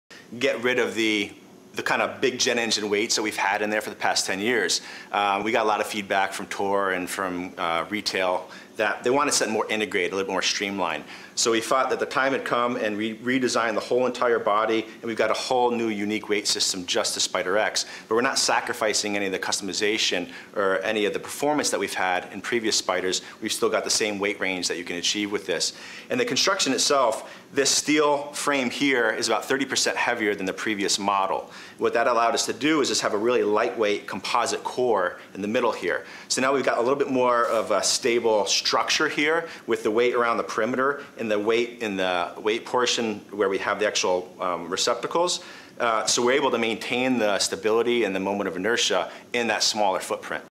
TaylorMade Spider X putter weighted differently for 2019
TaylorMade Spider X putter weighted differently for 2019. From Golf Talk Canada on TSN.